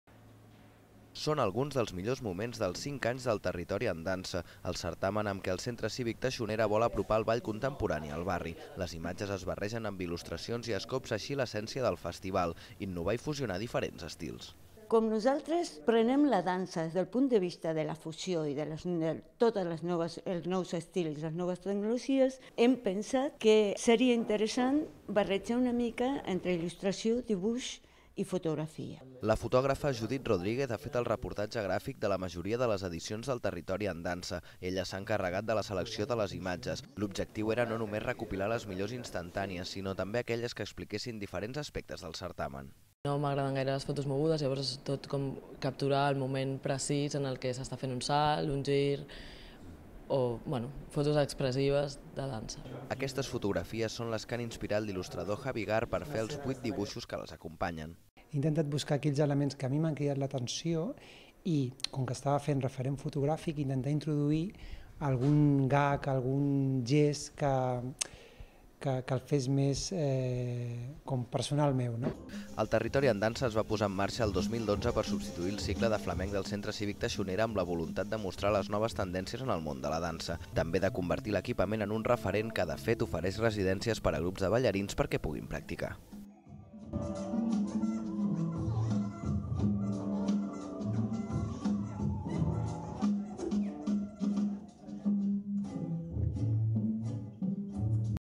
2017 04 24 Expo territori dansa
Aquest any el certamen Territori en Dansa arriba a la cinquena edició i des de l’equipament que l’organitza, el Centre Cívic Teixonera, ho han volgut celebrar amb una exposició. Com que el cicle se centra en la fusió i en la innovació, han volgut que la mostra també barregés diferents vessants artístiques. Per això han ajuntat les millors imatges que ha fet la fotògrafa Judith Rodríguez amb unes il·lustracions fetes expressament per a l’ocasió del dibuixant Javigaar.